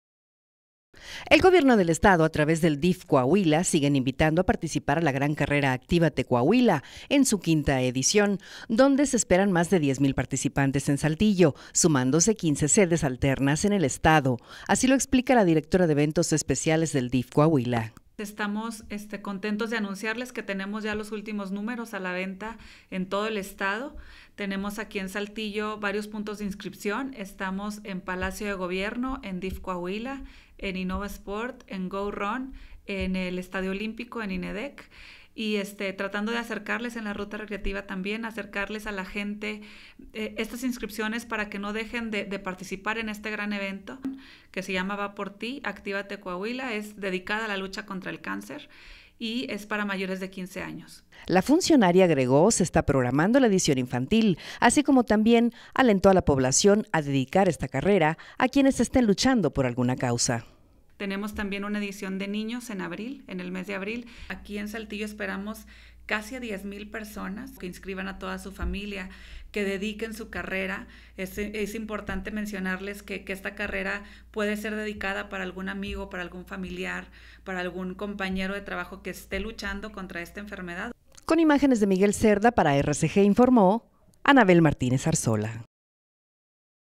Últimos boletos de registro ACTIVATE Coah 2016
El gobierno del estado a través del DIF Coahuila siguen invitando a participar en la gran carrera ACTIVATE Coahuila en su quinta edición, donde se esperan mas de diez mil participantes en Saltillo, sumándose 15 sedes alternas en el estado, así lo explica la directora de eventos especiales del DIF Coahuila.